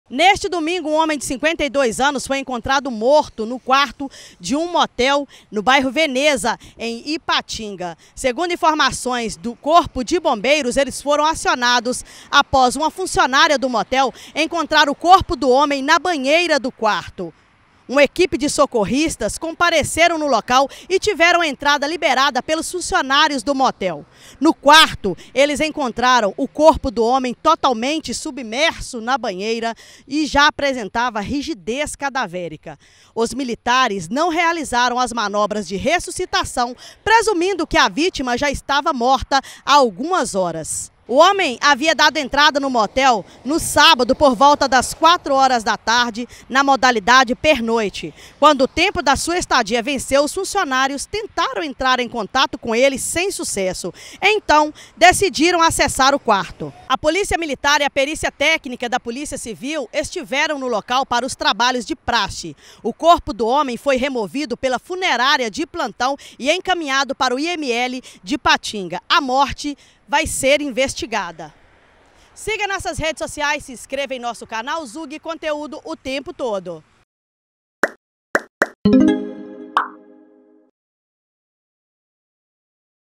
Homem é encontrado morto em quarto de motel em Ipatinga
Neste domingo (14), um homem foi encontrado morto em um quarto de motel, no bairro Veneza, em Ipatinga. O corpo do homem, de 52 anos, estava em uma banheira. Veja os detalhes:

👉 𝗖𝗼𝗻𝗳𝗶𝗿𝗮 𝗮𝘀 𝗿𝗲𝗽𝗼𝗿𝘁𝗮𝗴𝗲𝗻𝘀 𝗲𝗺 𝗻𝗼𝘀𝘀𝗼 𝘀𝗶𝘁𝗲.
👍 𝗖𝗹𝗶𝗾𝘂𝗲 𝗻𝗼 𝗹𝗶𝗻𝗸 𝗻𝗮 𝗕𝗜𝗢!